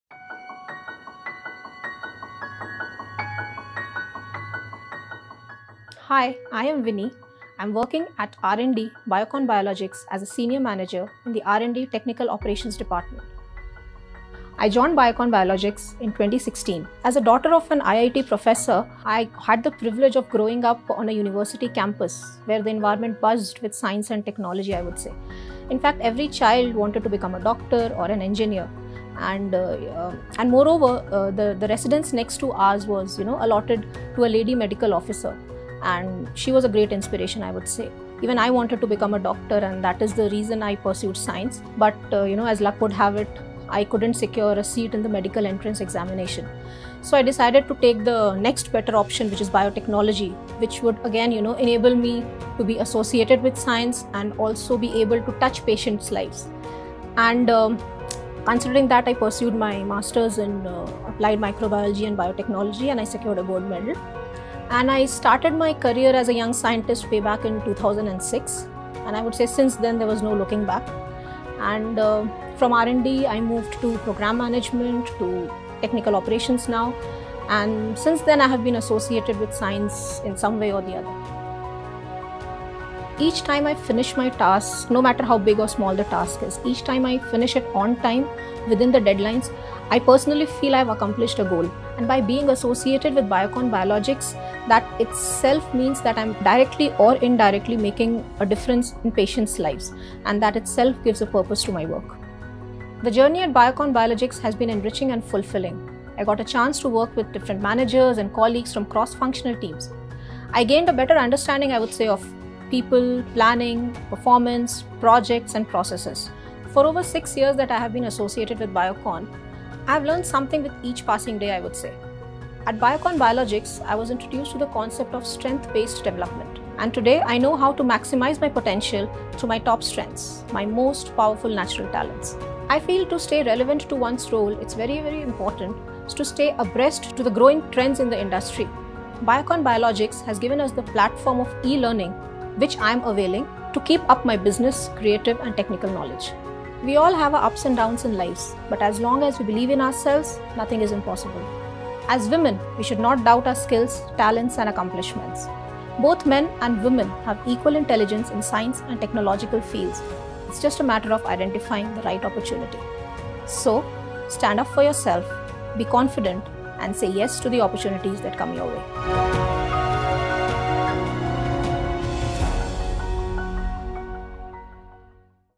SheInspires: The journey of Winny, Sr. Manager, Research & Development at BioconBiologics
Growing up on an IIT campus, Winny always wanted to become a doctor. She did not give up her dream of pursuing science when she couldn't clear the medical entrance exam. "My work at BioconBiologics gives me a purpose," says Winny. Watch her story in SheInspires